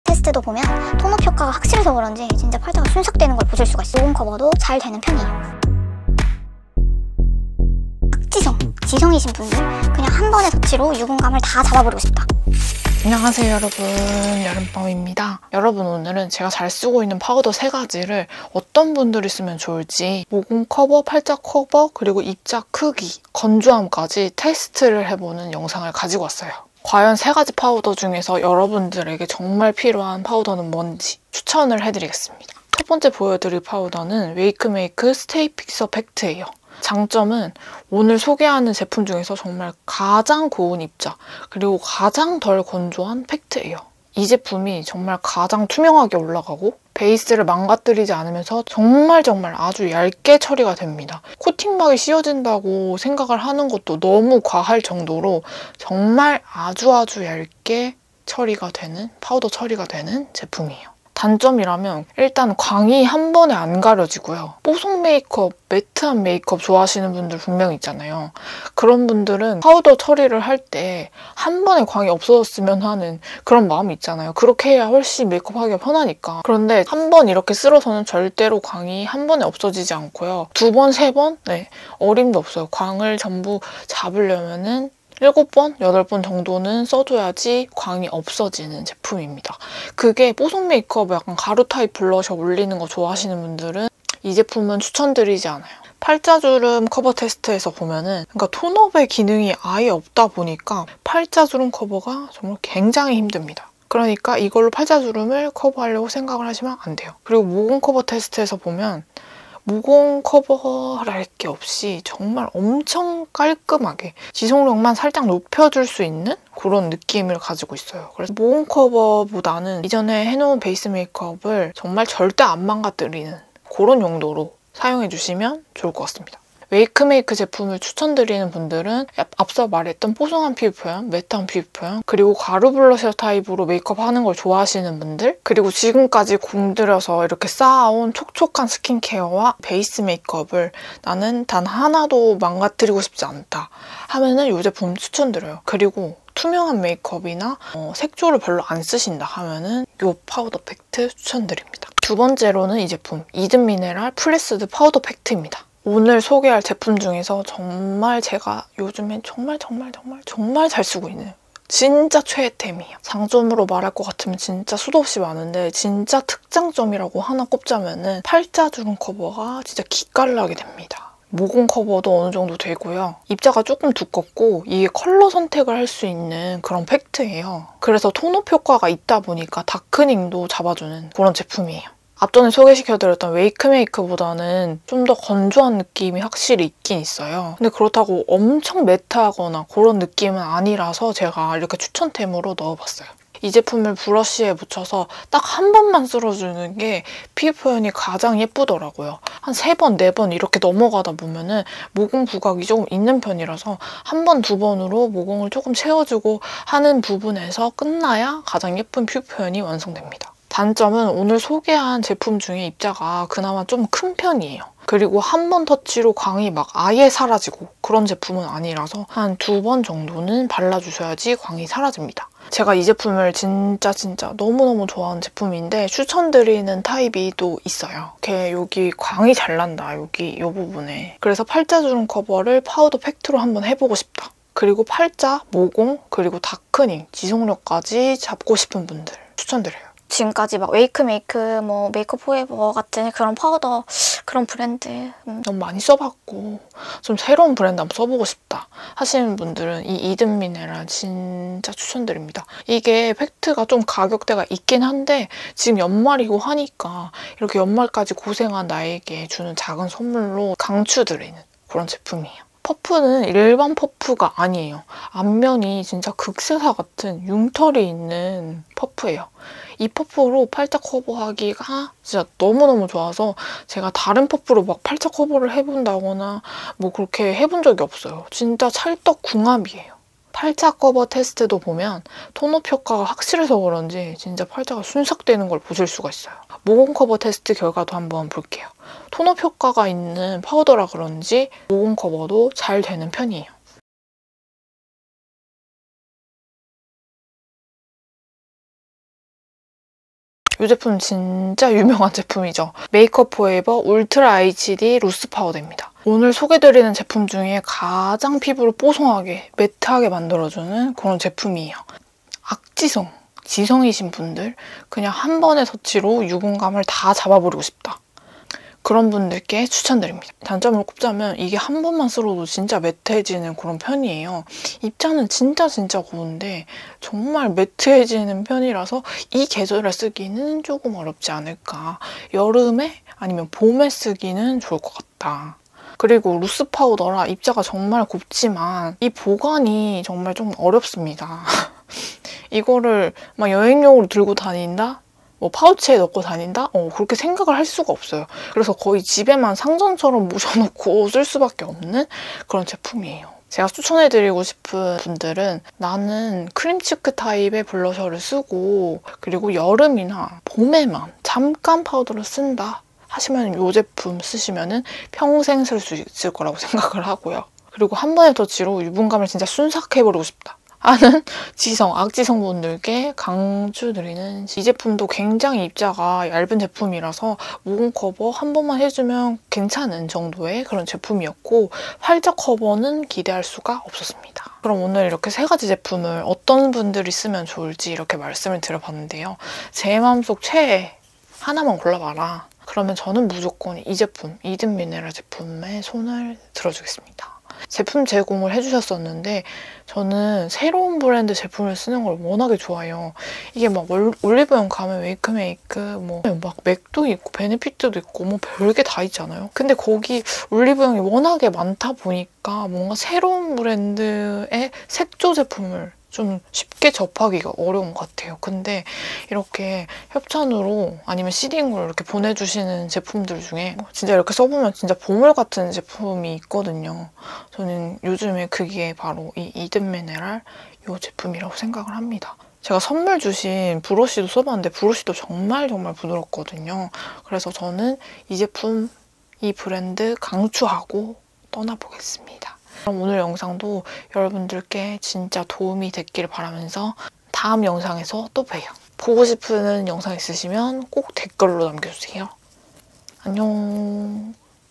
모공&팔자 순삭 파우더‼️건조함까지 테스트한 파우더 추천👍🏻
여러분 오랜만에 와서 놀라셨죠!😳

여행을 다녀오느라 조금 늦어졌어용✈️
앞으로는 다시 열심히
영상공장🎬🎥영업해볼게요🤍

오늘 영상은 모공도 뽀송메컵도 둘 다
사수하고 싶은 여러분들께 바치는 영상!

당연히 촉촉메컵이
모공커버엔 더 좋지만🤓

저는 요즘에 가루타입 블러셔가 너무 좋더라고요..
그래서 모공을 좀 포기하고
최대한 좋은 파우더는 뭘까 하고🤔🤔

여러가지 써본 중에
좋은 제품과 장단점! 소개합니당✨

0:00~0:32 인트로
0:33~2:31 웨이크메이크
2:32~5:00 이든미네랄
5:01~6:29 메이크업포에버
6:30~7:35 탑1